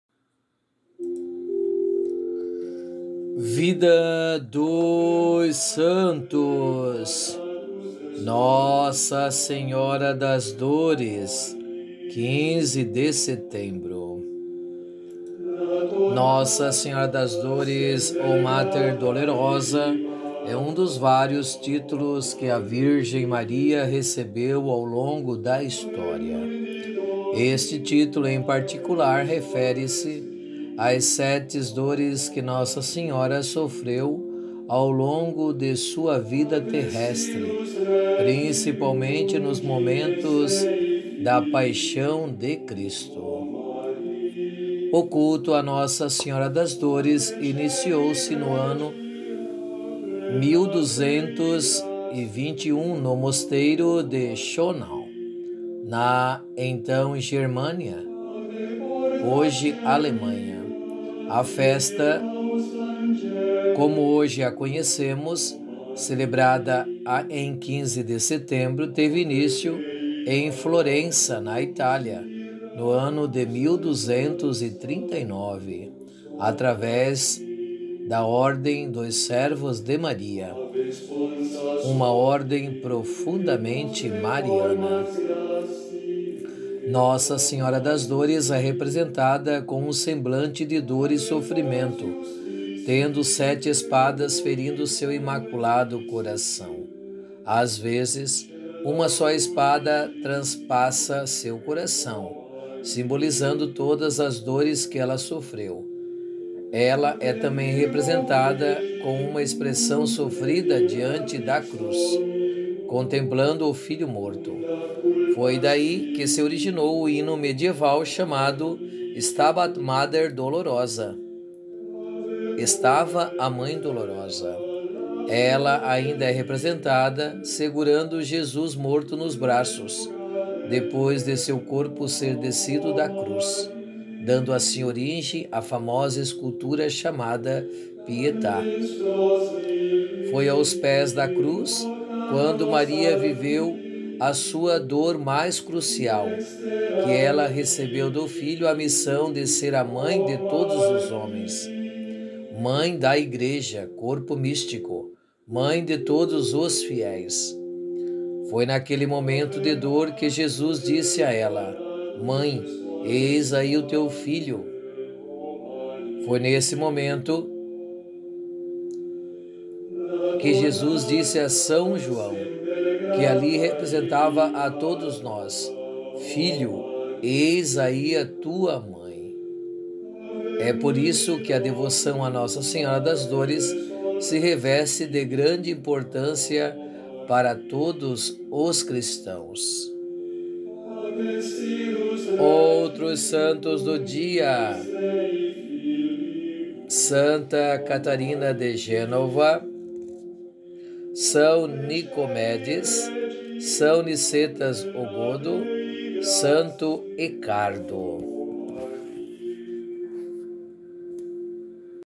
Vidas dos Santos - Nossa Senhora das Dores 15 Set
A devoção à “Mater Dolorosa”, muito difundida, sobretudo nos países do Mediterrâneo, desenvolveu-se a partir do final do século XI. Quer ser mais íntimo de Nossa Senhora leia o Tratado da Verdadeira devoção e faça sua consagração. Acesse @eloiziototustuus